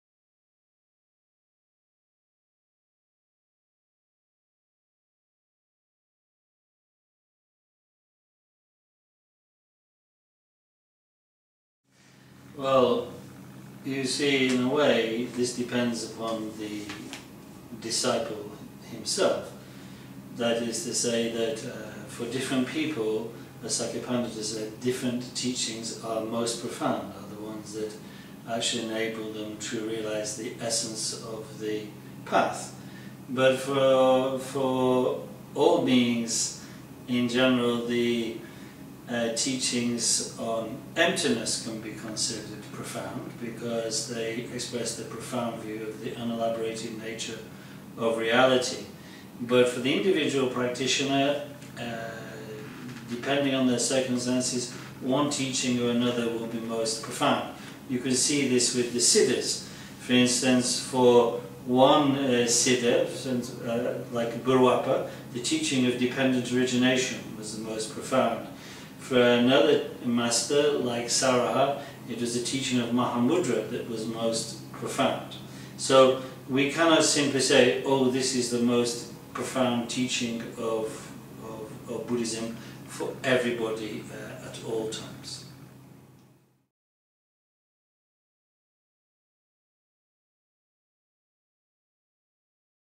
What is the most important teaching in Buddhism?
Lama Jampa Thaye talks about Buddhism. New York City, June 2012.